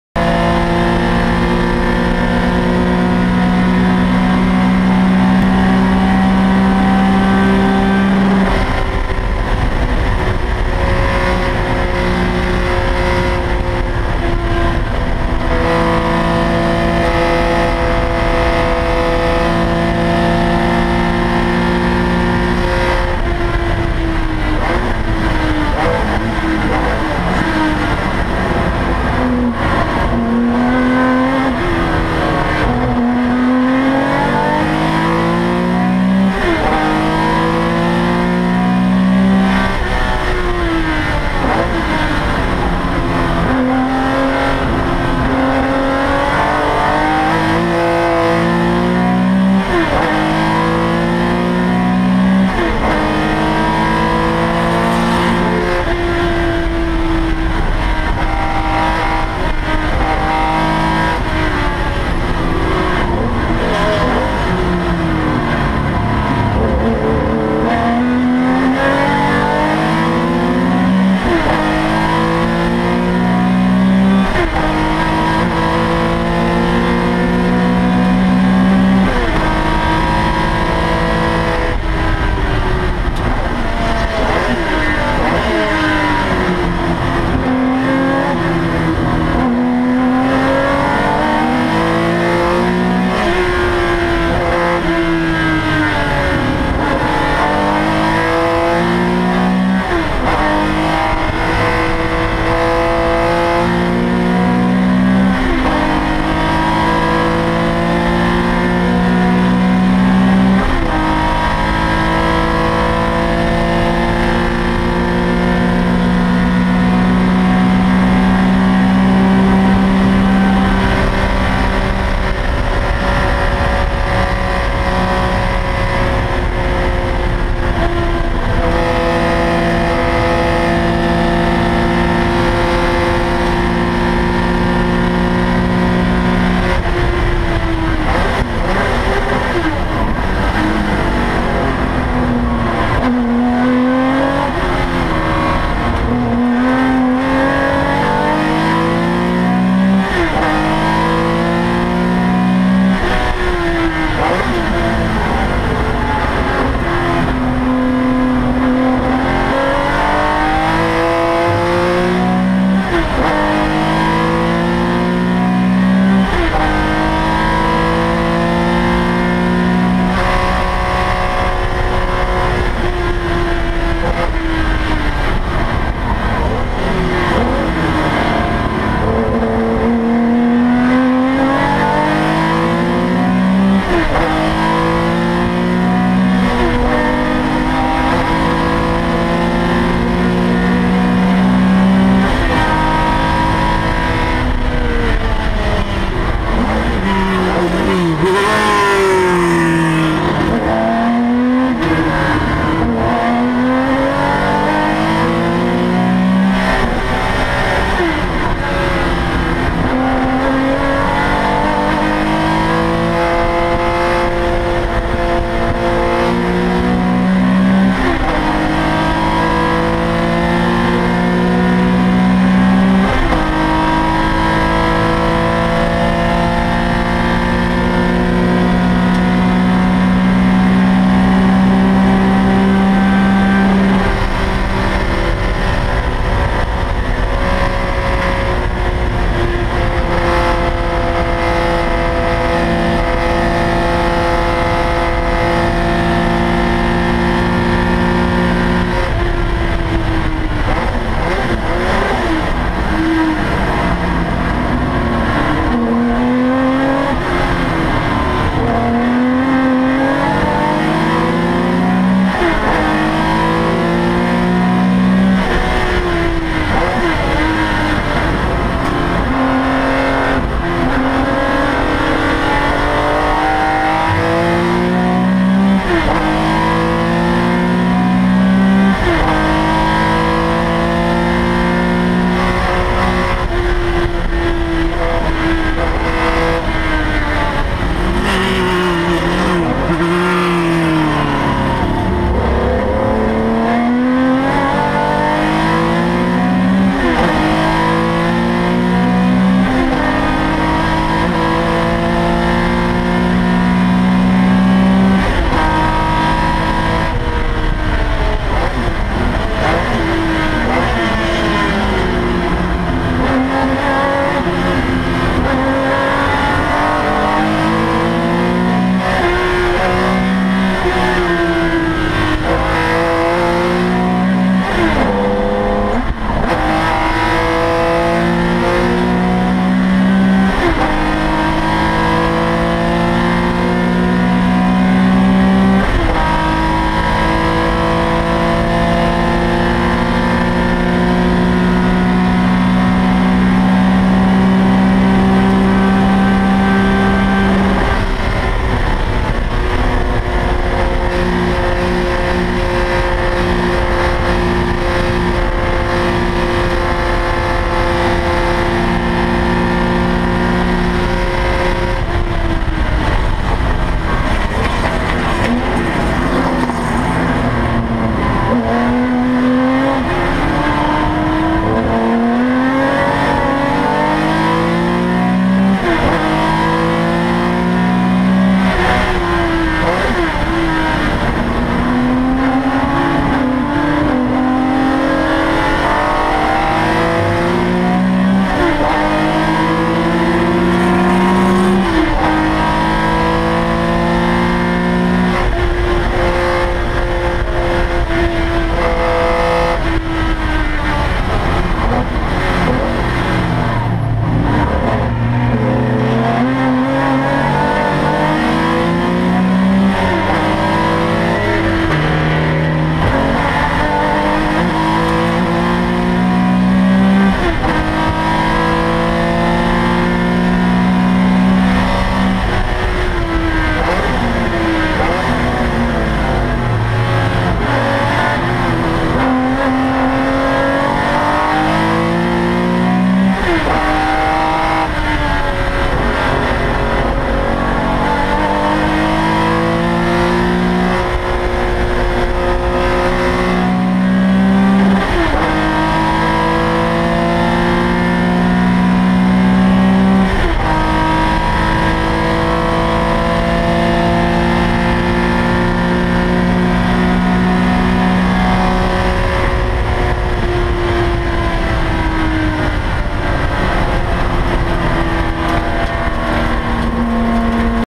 SVRA Historics @ Auto Club Speedway Sunday Race clip
The last few laps of SVRA's Sunday Race.  I got lucky on Saturday with a win, but Jeff wasn't going to make it easy on Sunday.  Jeff is a 2 time Daytona 24 winner (17 year, 24hr Veteran).  I was pushing hard till the very end.